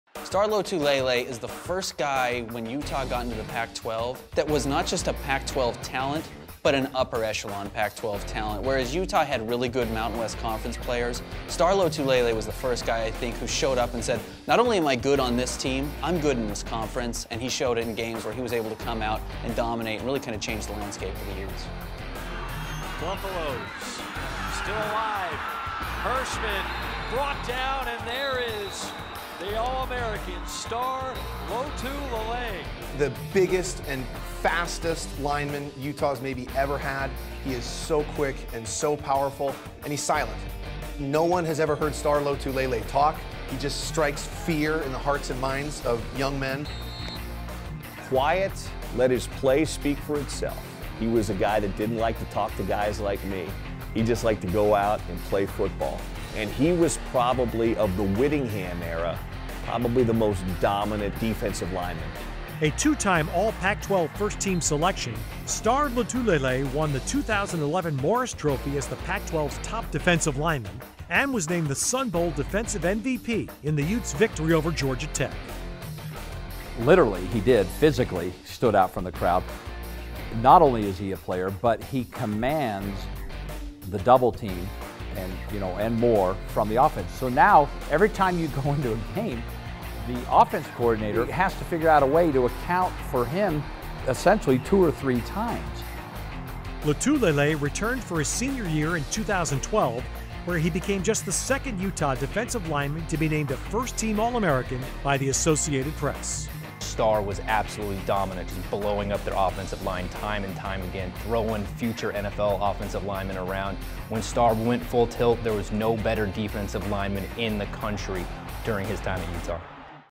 AAPI Heritage Month: The outstanding career of Star Lotulelei
Pac-12 Networks celebrates Asian American and Pacific Islander Heritage Month in this special feature of former Utah student-athlete Star Lotulelei. Lotulelei was drafted in the first round of the 2013 NFL Draft to the Carolina Panthers and has played in the NFL ever since. Follow Pac-12 sports with the Pac-12 Now App. Pac-12 Now is available today in your app store for iOS, Android, and Apple TV.